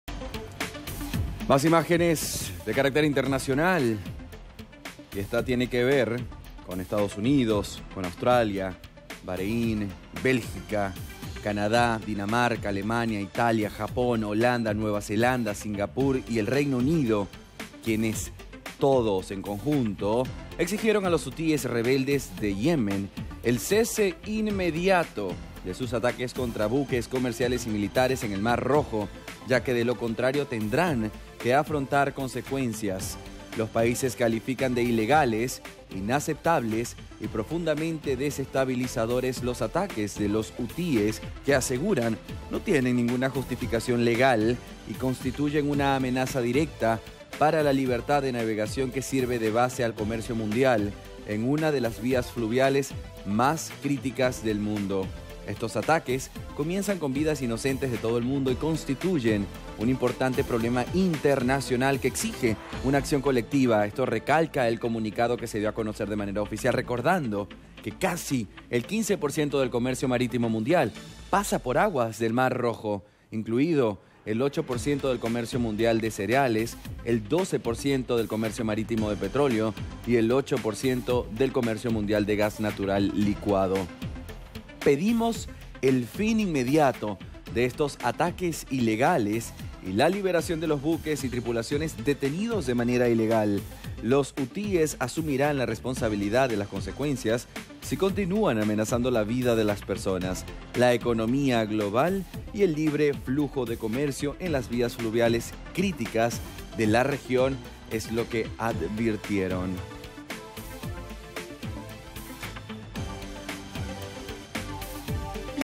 Estados Unidos y otros 12 países lanzan advertencia a los hutíes de Yemen
👉 EE.UU., Australia, Baréin, Bélgica, Canadá, Dinamarca, Alemania, Italia, Japón, Holanda, Nueva Zelanda, Singapur y el Reino Unido exigieron a los hutíes rebeldes de Yemen el cese "inmediato" de sus ataques contra buques comerciales y militares en el mar Rojo, ya que, de lo contrario, tendrán que afrontar "consecuencias".